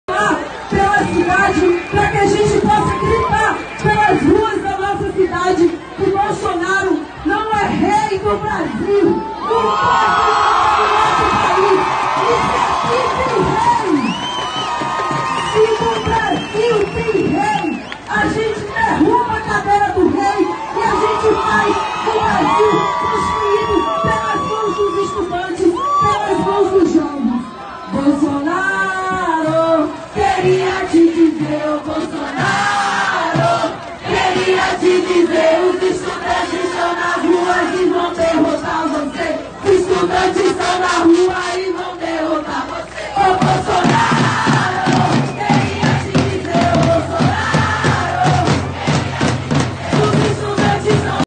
Ato em São Paulo contra os cortes na Educação
30 M no largo da Batata em Sao Paulo